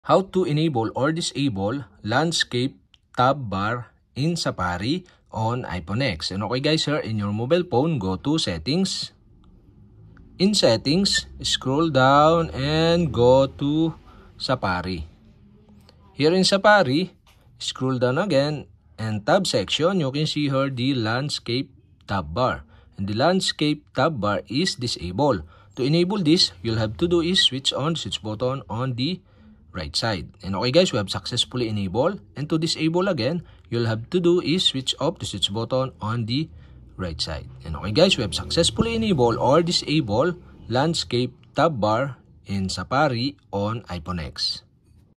How to Enable or Disable Landscape Tab Bar in Safari on iPhone X
How to enable or disable landscape tab bar in Safari on iPhone X mobile phone.